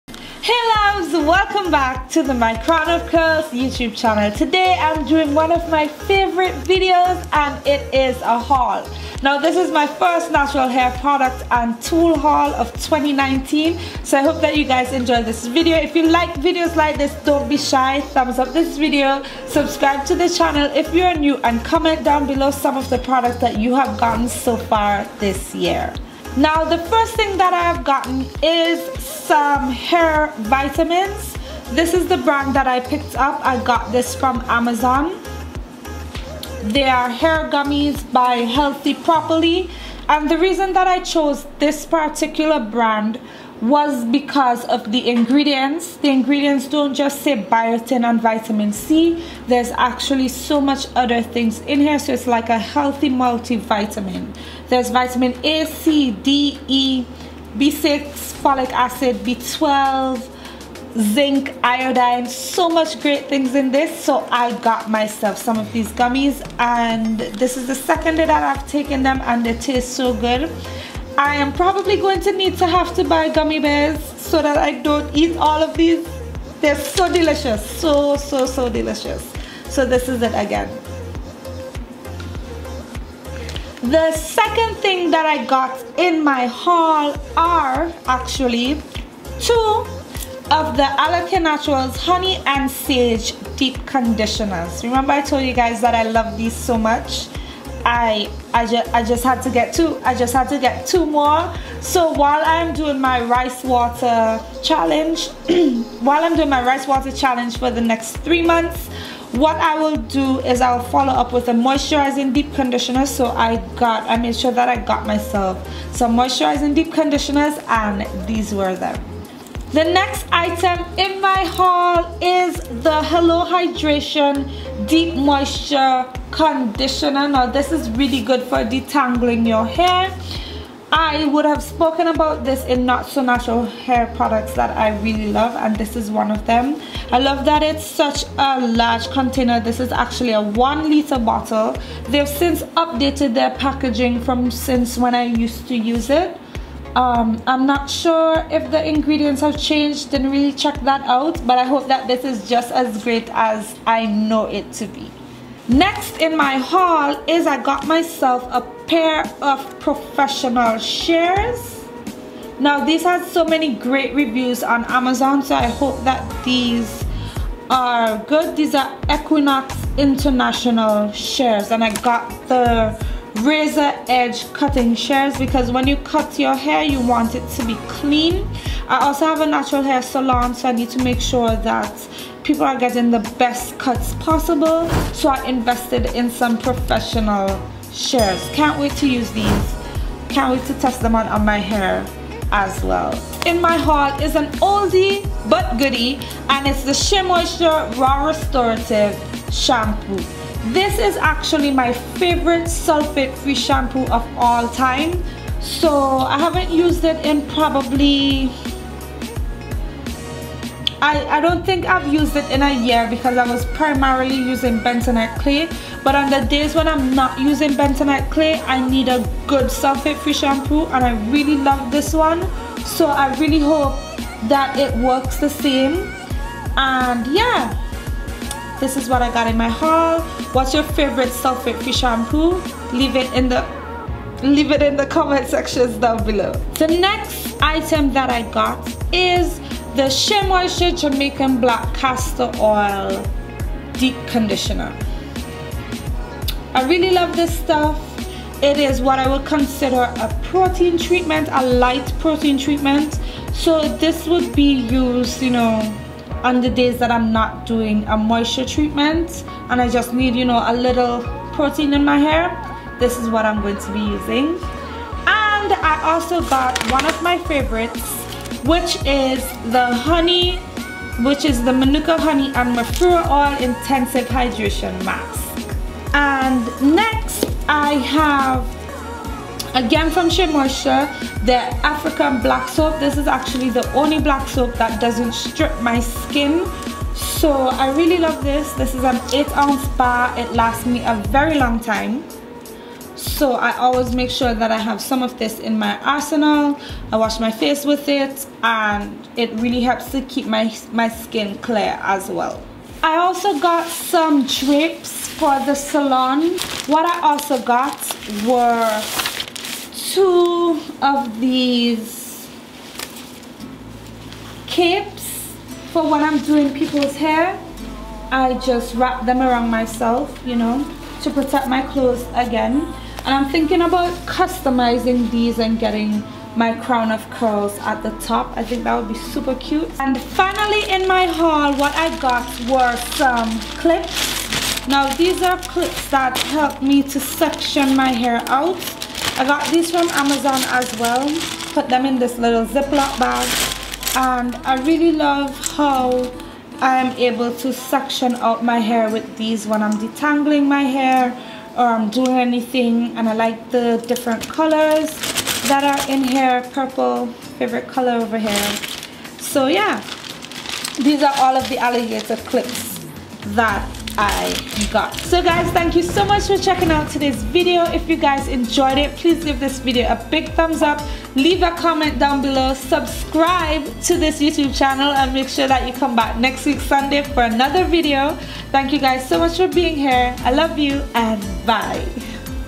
1ST Natural Hair Product & Tool Haul 2019
Welcome Back! Its my first natural hair product haul of 2019! In this haul we have hair and skin care products as well as tools that i hope to use this year.